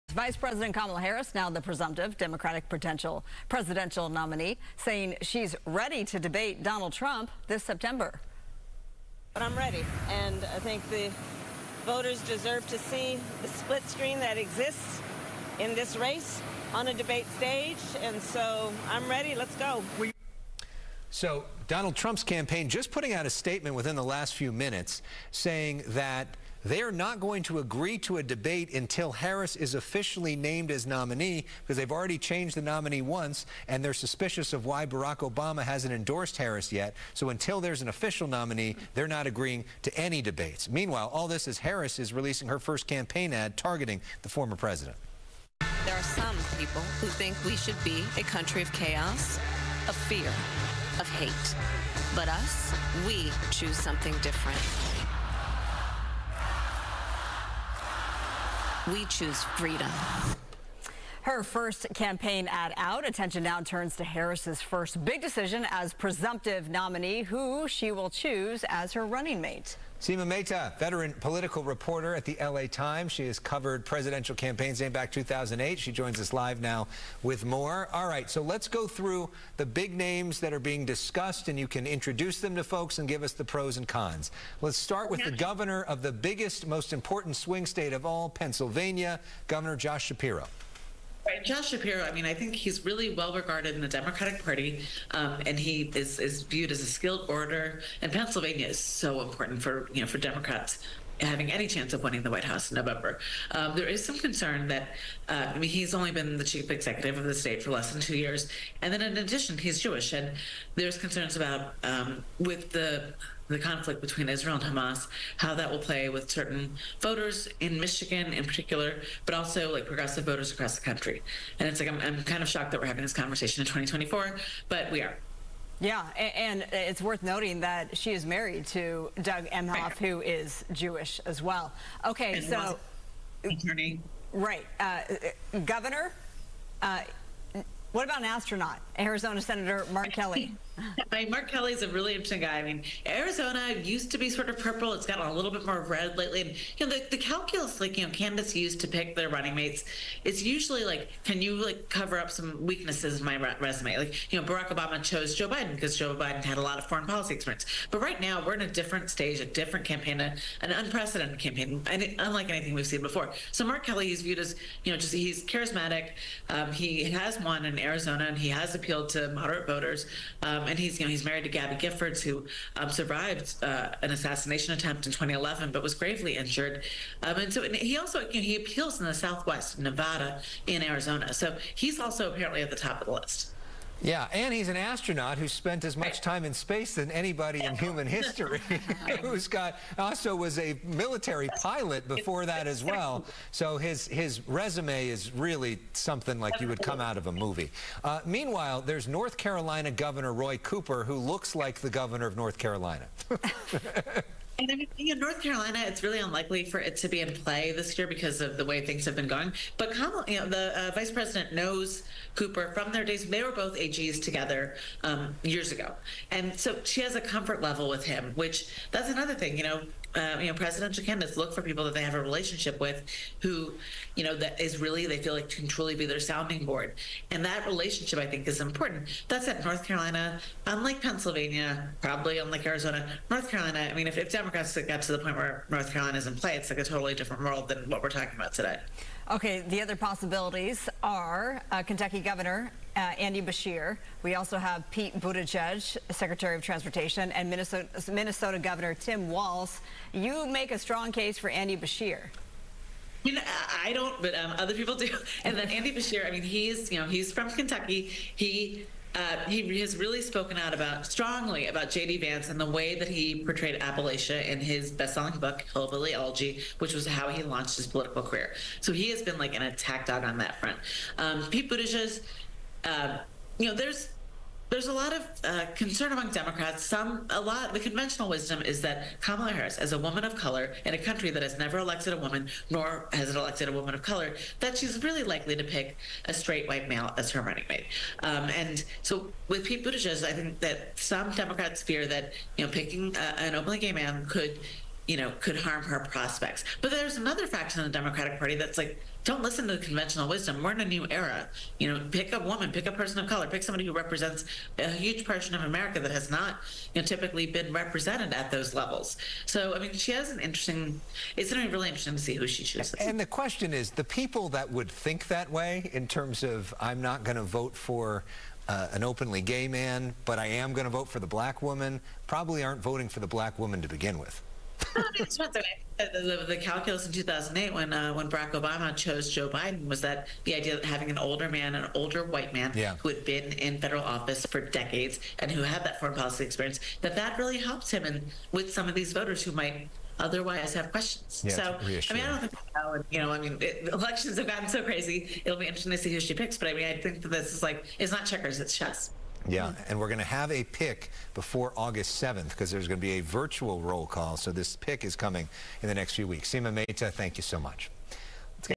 Who will Kamala Harris pick as her VP?
Harris' campaign just dropped their first ad and the presumptive Democratic nominee said she's ready to debate Trump in September. But many are still wondering who she'll pick as her vice president.